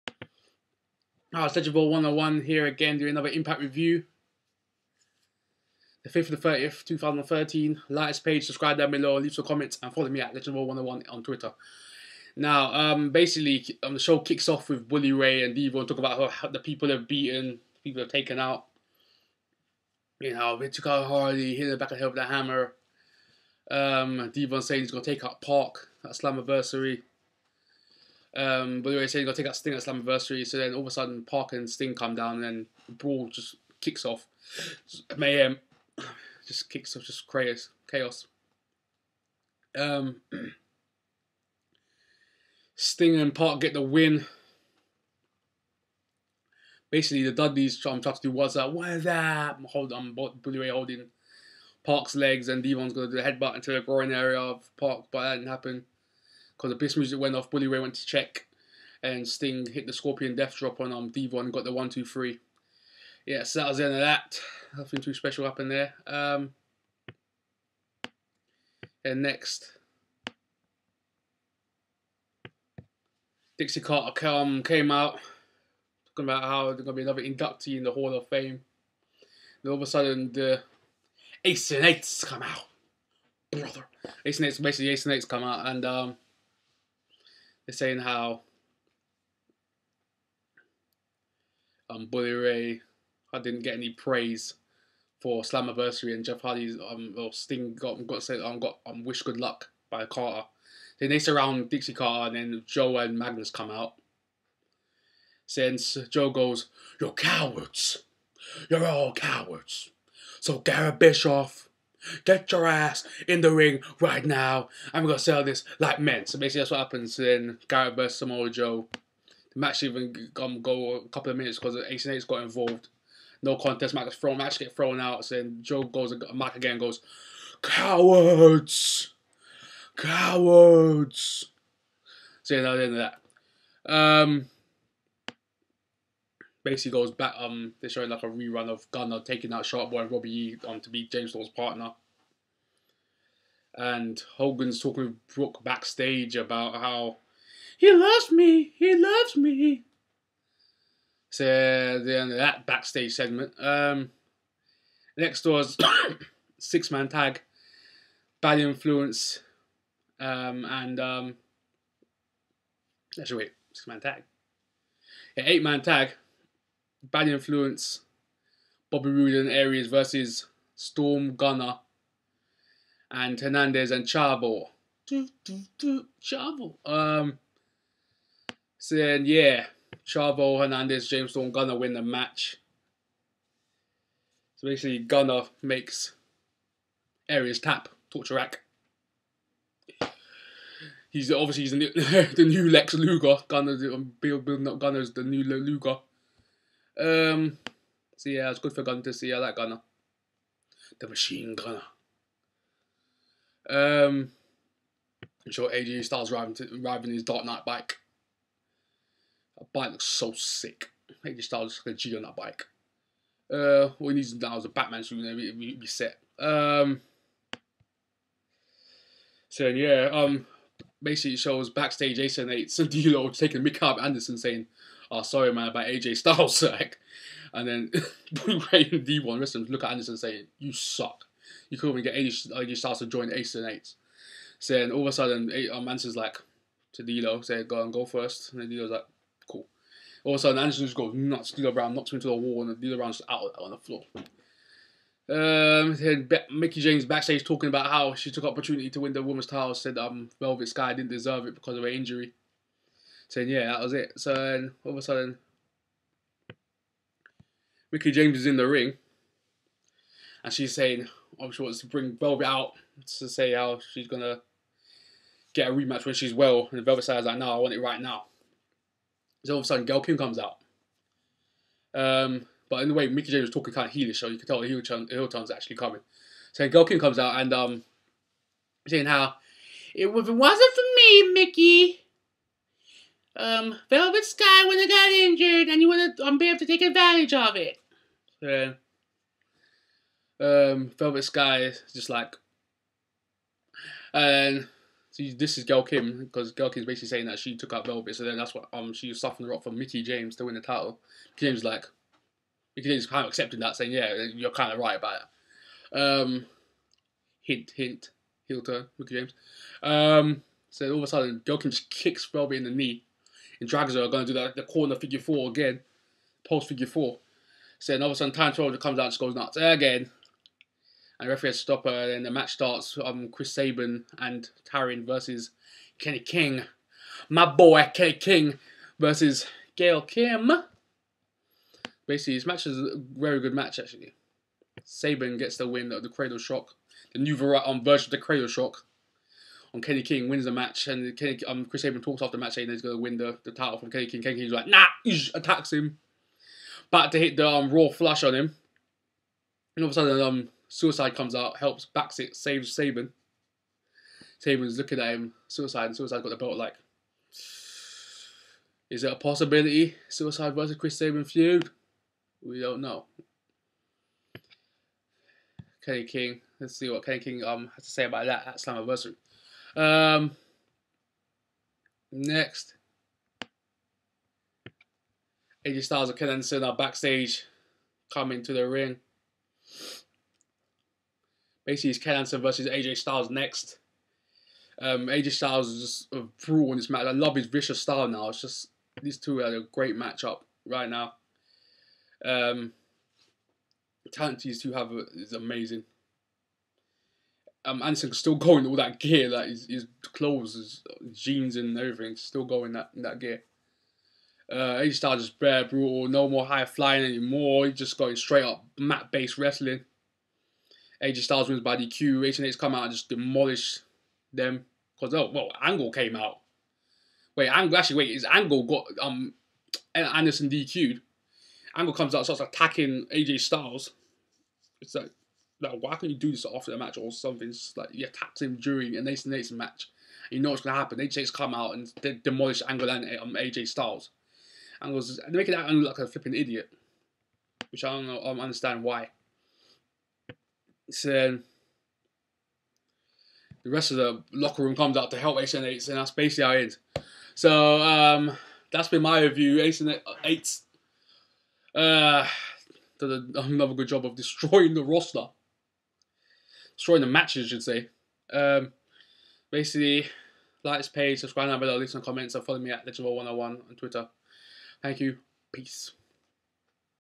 Impact 5/30/13 Review: Team 3D Reunite.
Team 3D take on Sting and Joseph Park.